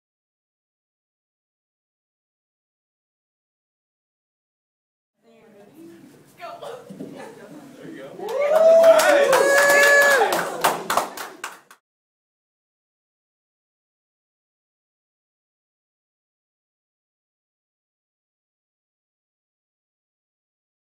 Yoga 101 Bad Yoga Airplane
A fun day learning how to do the Yoga Airplane.